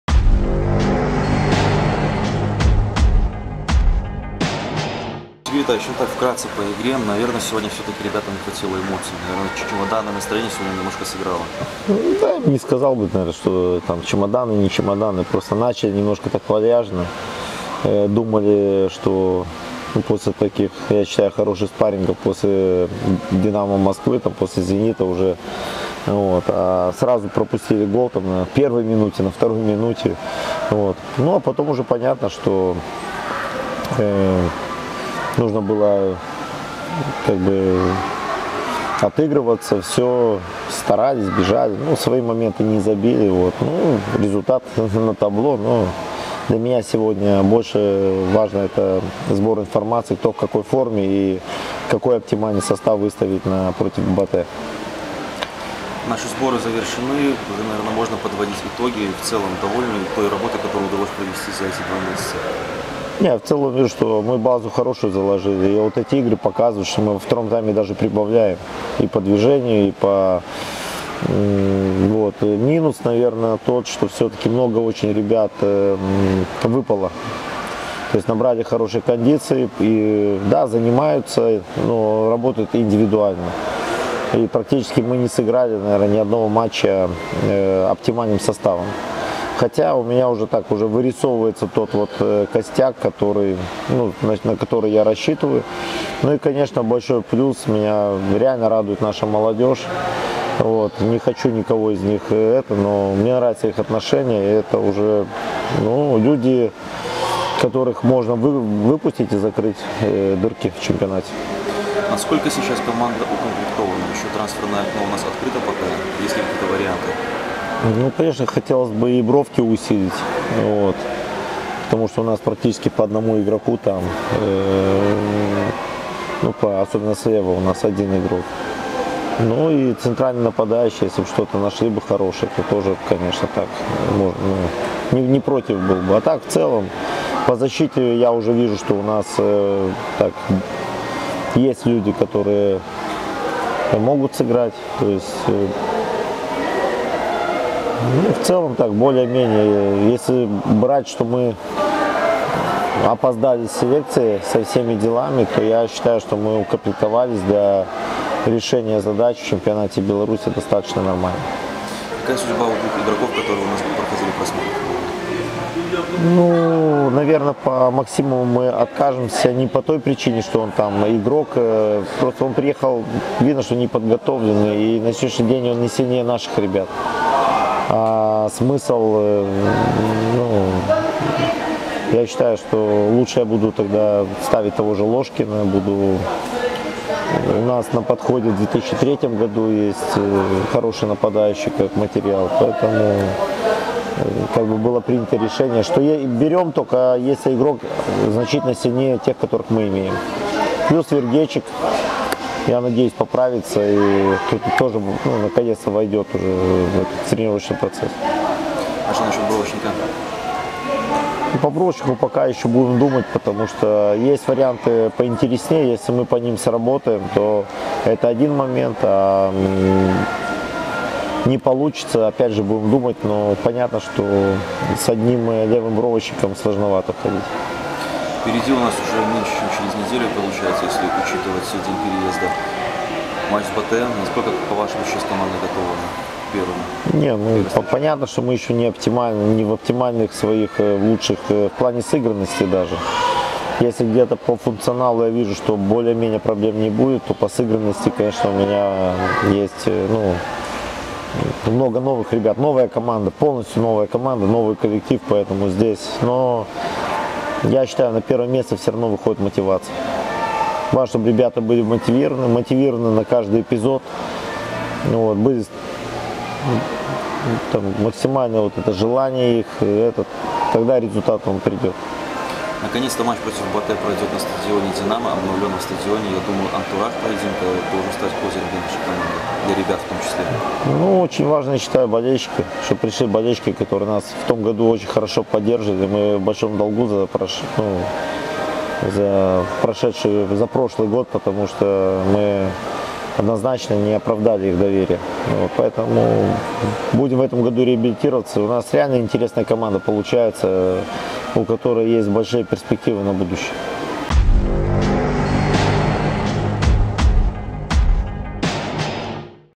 Итоги матча против "Спартакс"
Комментарии Сергея Гуренко по итогам заключительного матча сбора